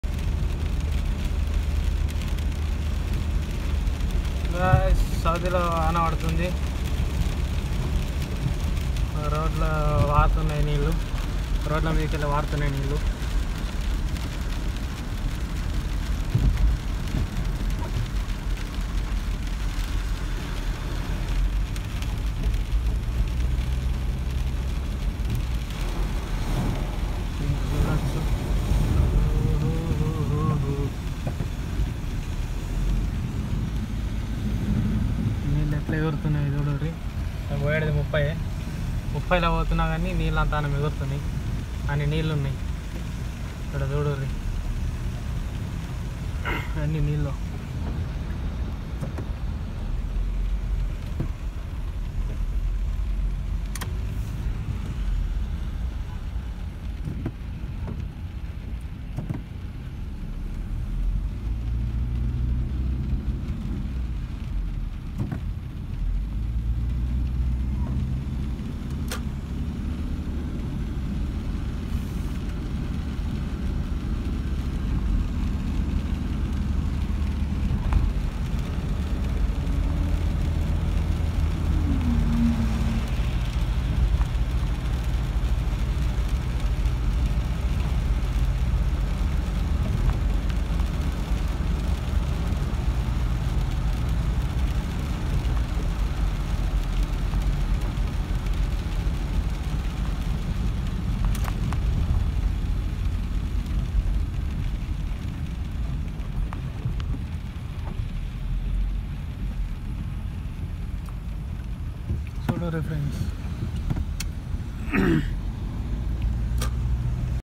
Rain in Saudi Riyadh | Telugu | India
Raining in Riyadh Saudi
Telugu people in Saudi